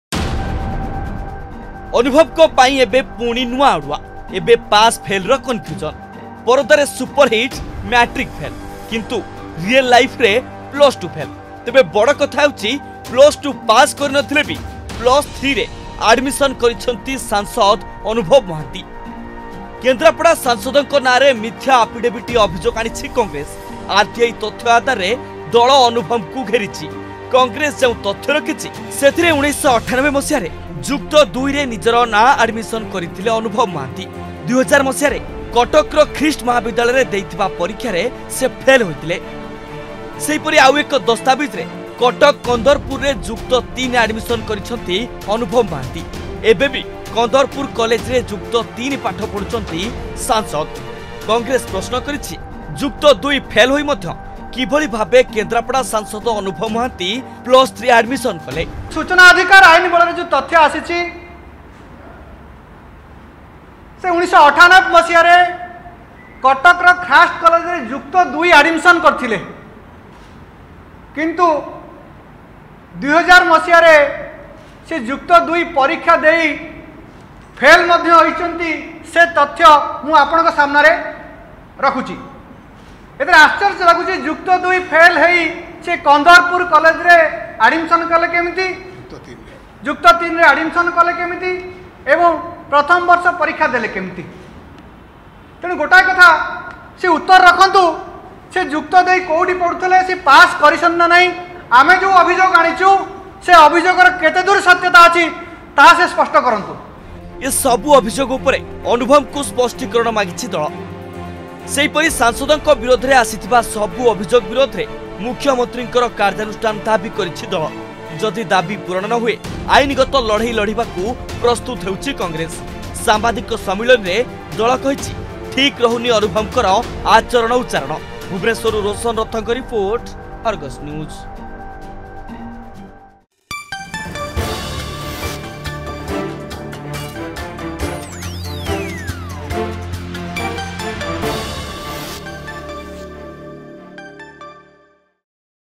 ଅନୁଭବଙ୍କ ପାଇଁ ନୂଆ ଅଡ଼ୁଆ; ଏବେ ପାସ୍‌-ଫେଲ୍‌- କନଫ୍ୟୁଜନ
ପରଦାରେ ଅନୁଭବଙ୍କୁ ସଫଳତା ଦେିଥିଲା ମାଟ୍ରିକ ଫେଲ  । କିନ୍ତୁ ଏବେ ତାଙ୍କ ଯୁକ୍ତ ଦୁଇ ଫେଲକୁ ନେଇ ଚର୍ଚ୍ଚା  । କାରଣ ଆରଟିଆଇ ତଥ୍ୟ ଆଧାରରେ ସାଂସଦଙ୍କ ବିରୋଧରେ କଂଗ୍ରେସ ଆଣିଛି ଅଭିଯୋଗ  । ବିନା +2 ପାସରେ ଯୁକ୍ତ ତିନିରେ ଆଡମିସନ ନେଇଛନ୍ତି ବାଳୁଙ୍ଗା ଟୋକା  । ଦଳ ପ୍ରଶ୍ନ କରିଛି, ମିଛ ଆଫିଡେବିଟ କରିଥିବାରୁ ନିଆଯାଉ ଆଇନଗତ କାର୍ଯ୍ୟାନୁଷ୍ଠାନ  ।